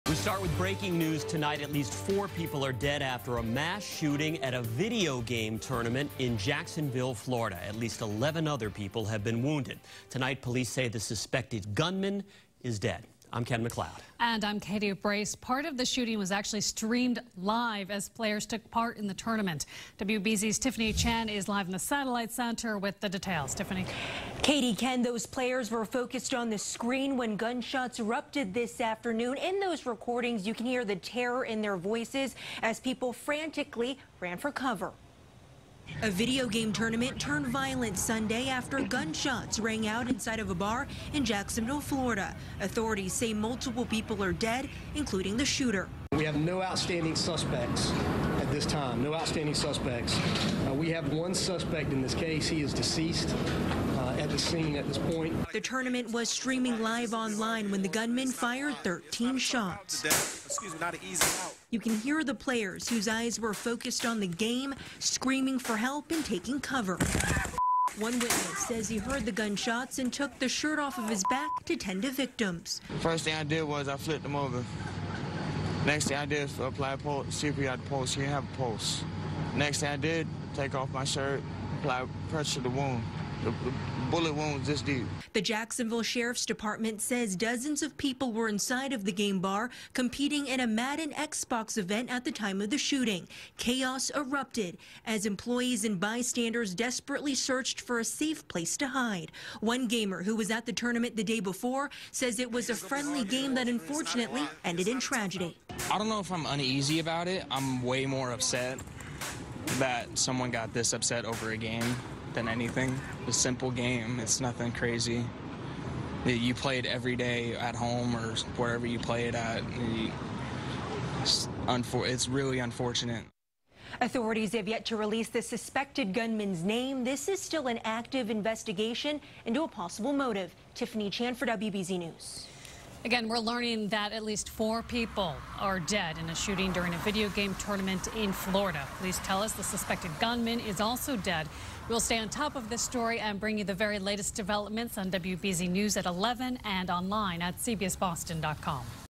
Shooting At Jacksonville 'Madden' Video Game Tournament Leaves At Least 4 Dead
A gunman opened fire Sunday at an online video game tournament that was being livestreamed from a Florida mall, killing multiple people and sending many others to hospitals, authorities said. WBZ-TV's Tiffany Chan reports.